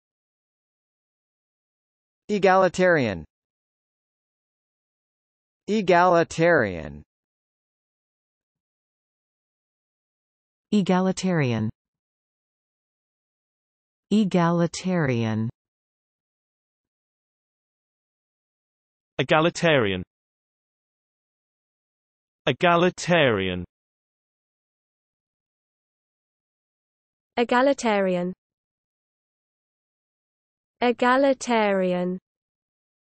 How to pronounce egalitarian
Egalitarian definitions:
    (n): a person who believes in the equality of all people
    (s): favoring social equality; "a classless society"